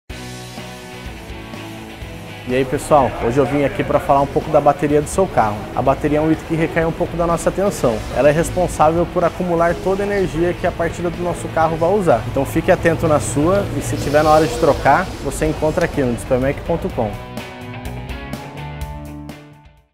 Dica rápida sobre bateria
A bateria é uma parte muito importante do carro, pois ela é responsável por acumular a energia da partida do seu veículo. Tenha sempre a revisão em dia e preste muita atenção a qualquer mudança na bateria do seu carro.

Se ligue também em outras curiosidades e informações sobre carros e autopeças no nosso blog